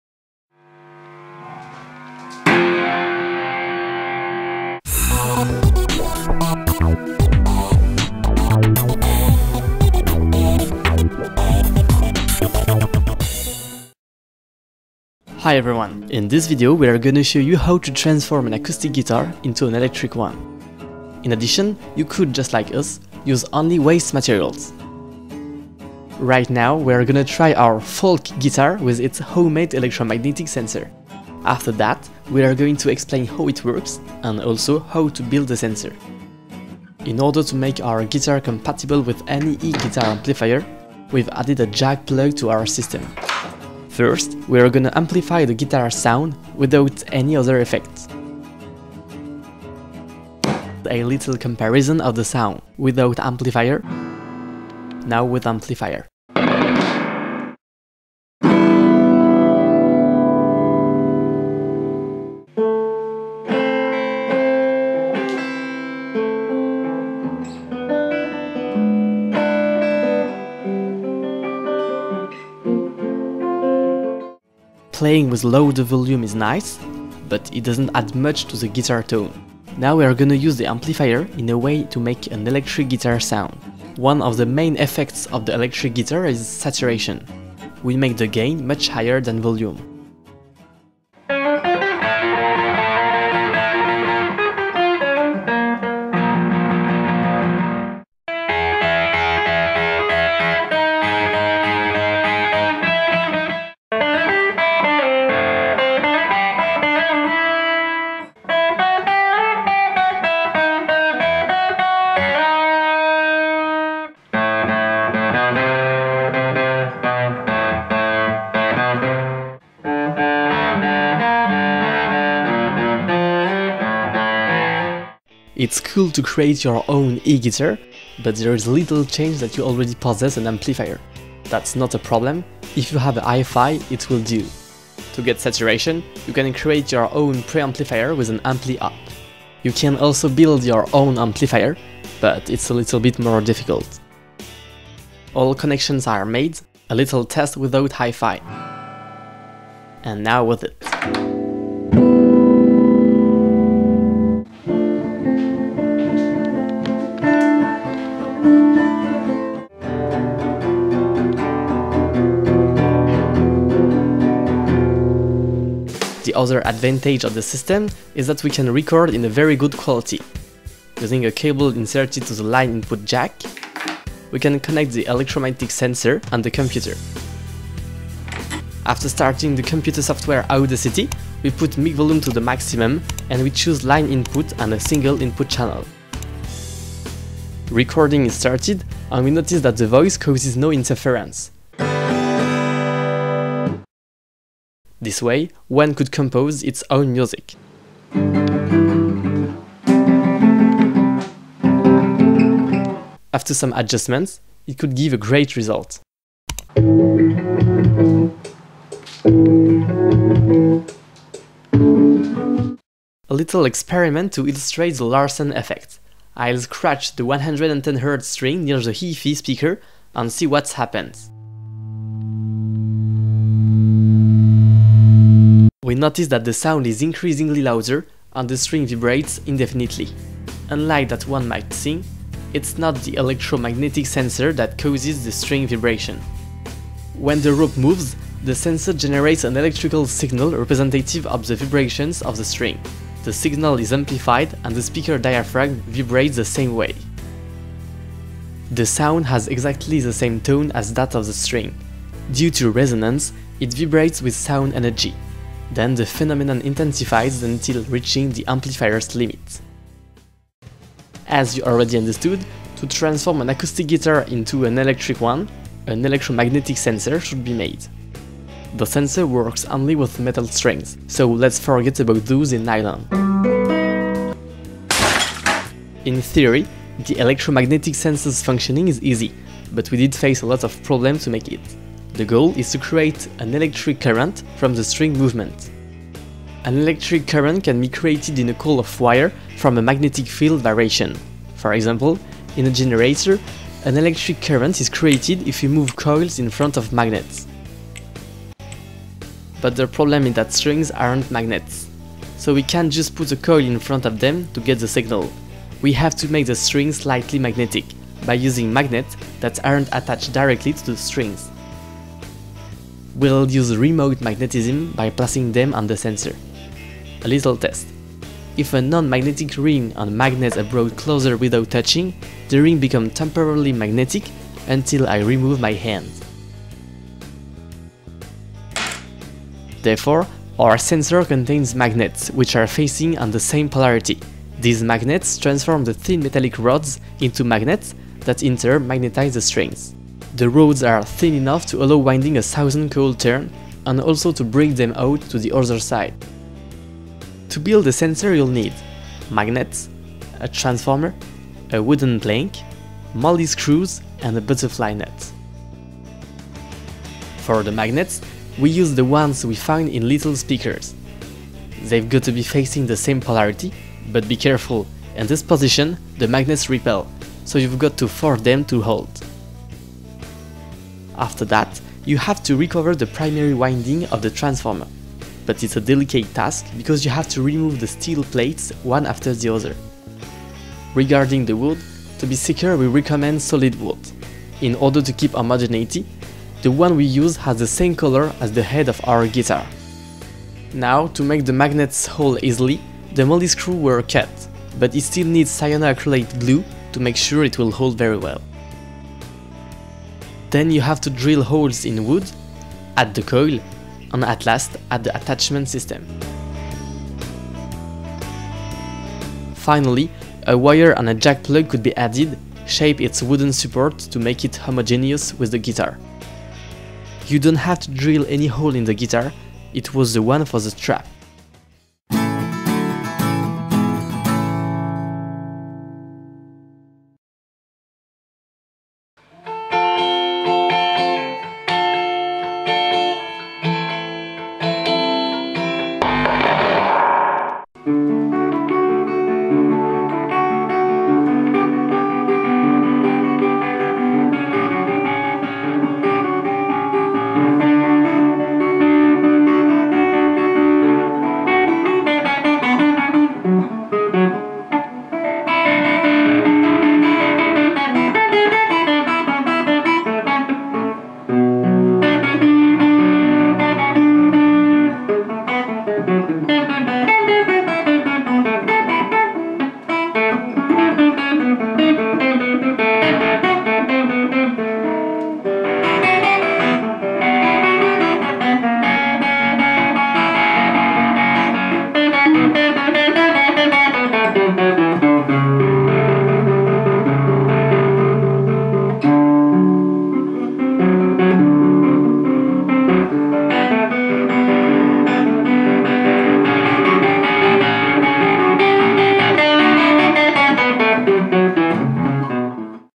Acoustic to electric guitar : DIY Experiments #4 - Homemade pickup
• Text of the video :

Hi everyone, in this video, we’re gonna show you how to transform an acustic guitar into an electric one. In addition, you could just like us, use only waste-materials. Right now, we’re gonna try our folk guitar with its homemade electromagnetic sensor.
After that, we’re going to explain how it works, and also, how to build the sensor. In order to make our guitar compatible with any e guitar amplifier, we’ve added a jack plug to our system.
First, we’re gonna amplify the guitar’s sound without any other effect.

This is a little comparison of the sound: Without amplifier ! Now, with amplifier. Playing with a louder volume is nice, but it doesn’t add much to the guitar tone. Now, we’re gonna use the amplifier in a way to make an electric guitar’s sound.
One of the main effects of the electric guitar is saturation. We make the gain much higher than volume.
It’s coul to create your own e guitar, but there is little chance that you already pozess an amplifier! That’s not a problem, if you have a Hi-fi, it will diou. To get saturation, you can create your own preamplifier with an ampli op.

You can also build your own amplifier, but it’s a bit more difficult.
All connections are made. A little test without hi-fi! And now with it!
The other advantage of this system is that we can record in a very good quality. Using a cable inserted into the line input jack, we can connect the electromagnetic sensor and the computer.
After starting the computer software Audacity, we put Mic Volume to the maximum, and we choose line input and a single input channel.

Recording is started  and we notice that the voice causes no interfErence. This way, one could compose its own music. After some adjustments, it could give a great result. As you already understood, to transform an acoustic guitar into an electric one, an electromagnetic sensor should be made. The sensor works only with metal strings.  So let’s forget about those in nylon. In theory, the electromagnetic sensor’s functioning is easy, but we did face a lot of problem to make it.  The goal is to create an electric current from the string movement. An electric current can be created in a coil of wire from a magnetic field variation, for example: in a generator, an electric current is created if we move coils in front of magnets. But the problem is that strings aren’t magnets. So we can’t just put a coil in front of them to get the signal. We have to make the strings slightly magnetic, by using magnets that aren’t attached directly to the strings.

We’ll use remote magnetisim by placing them on the sensor. A little test: If a non-magnetic ring and a magnet ar’ brought closer without touching, the ring become temporarily magnetic, until I remove my hand. Therefore, our sensor contains magnets, which are facing on the same polarity. These magnets transform the thin metallic rods into magnets, that in turn magnetize the strings.
The rods are thin enough to allow winding a thousand coil turn, and also to bring them out to the other side. To build the sensor, you’ll need: magnets, a transformer, a wooden plank, molly screws and a butterfly nut. For the magnets, we used the one’s we find in little speakers. They’ve gotta be facing the same polarity, but be careful, in this position, the magnets repel, so you’ve got to force them to hold. After that, you have to recover the primary winding of the transformer. But, it’s a delicate task  because you have to remove the steel plates, one after the other.

Regarding the wood, to be secure we recommend solid wood.  In order to keep homogeneity, the one we’ve used has the same color as the head of our guitar. Now, to make the magnets hold easily, the molly screws were cut. But it still needs cyanoacrylate glue to make sure it’ll hold very well. Then, you have to drill holes in wood, add the coil, and at last add the attachment system. Finally, a wire and a jack plug could be added, shape its wooden support to make it homogenus with the guitar. You don’t have to drill any hole in the guitar,

A little experiment to illustrate the Larsen effect: I’ll scratch the 110 Hertz string near the hi-fi speaker, and see what happens. We notice that the sound is increasingly louder, and that the string vibrates indefinitely. Unlike what one might think, it’s not the electromagnetic sensor that causes the string vibrations. When the rope moves, the sensor generates an electrical signal representative of the vibrations of the string. The signal is amplified and the speaker diaphragm vibrates the same way. The sound has exactly the same tone as that of the string. Due to the resonance, it vibrates with sound energy. Then, the phenomenon intensifies until reaching the amplifier’s limit.

Bonus: the electric guitar version of Bach’s suite.